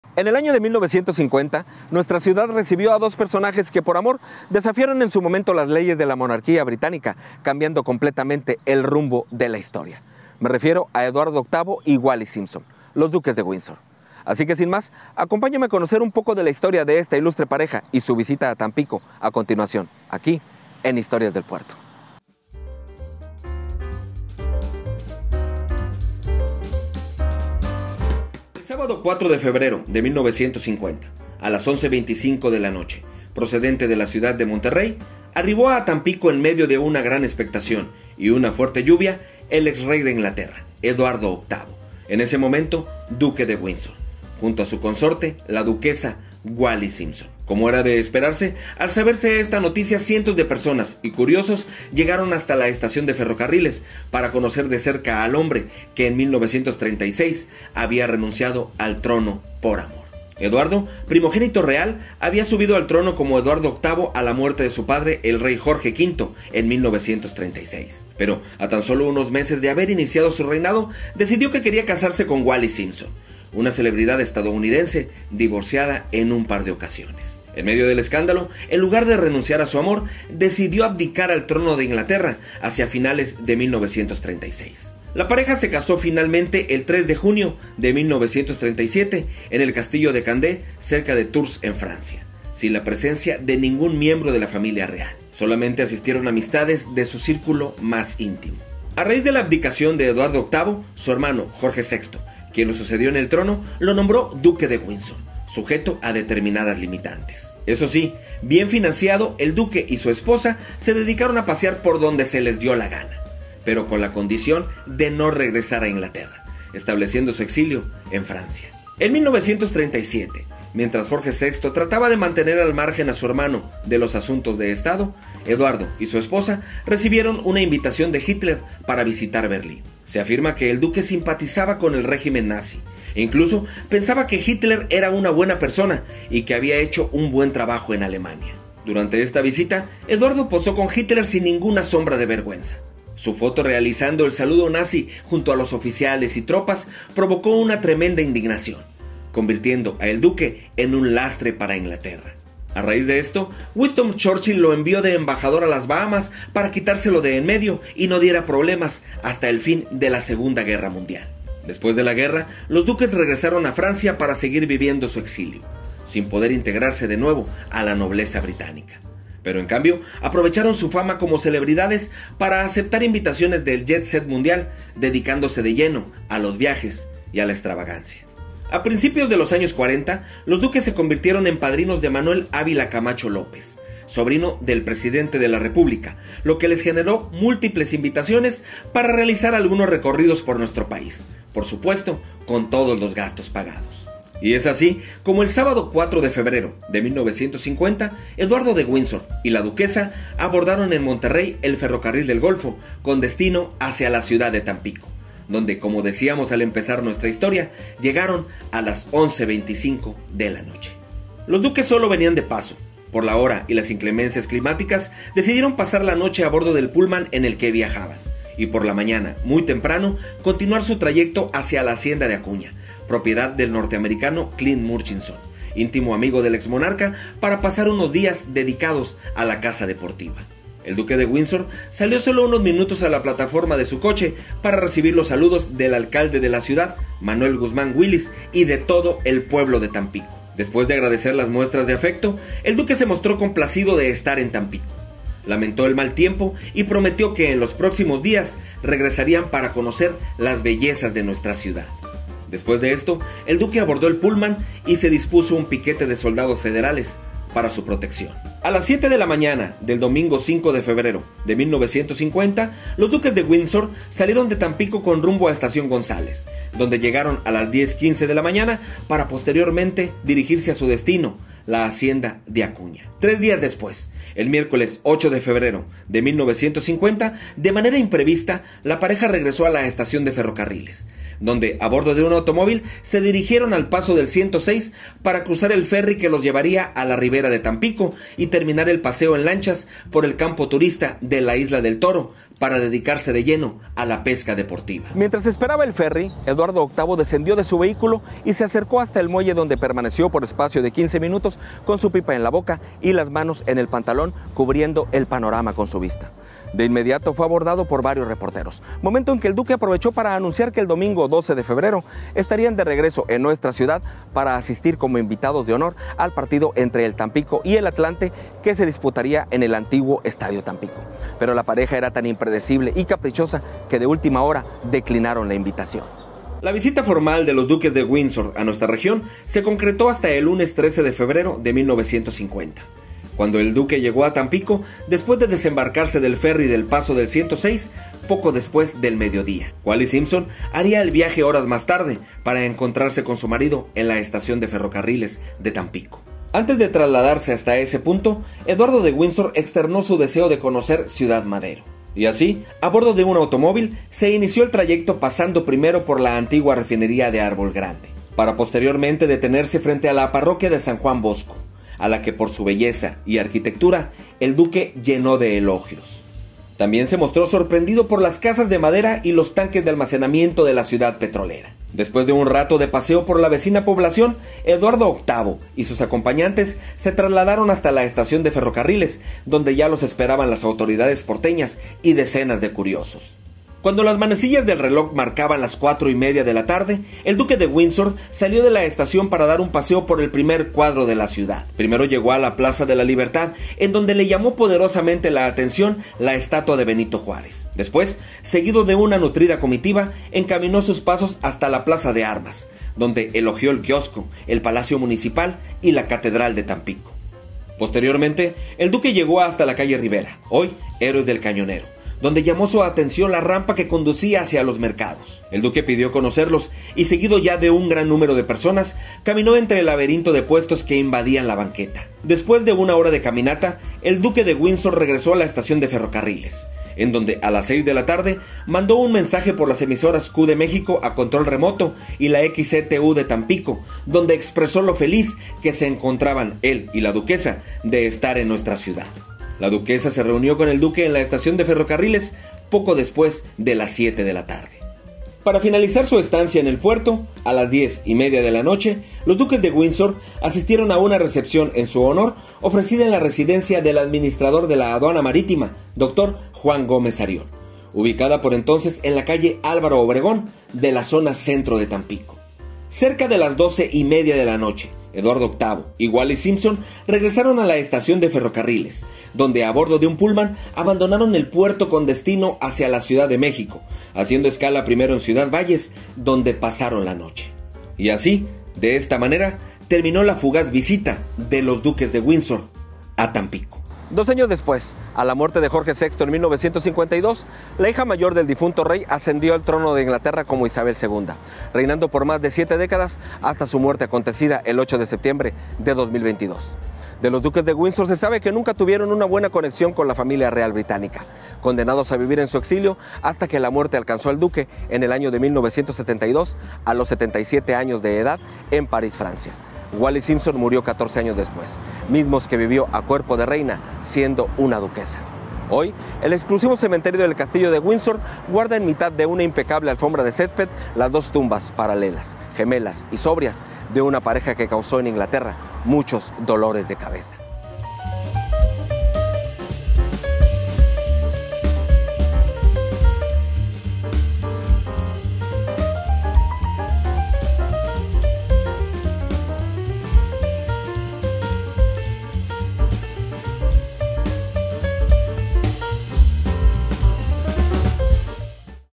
La visita de los Duques de Windsor a Tampico
En el año de 1950 nuestra ciudad recibió a dos personajes que por amor desafiaron en su momento las leyes de la monarquia británica cambiando completamente el rumbo de la historia.

La visita de los duques de Windsor a Tampico, una historia que contar aquí en Historias Del Puerto.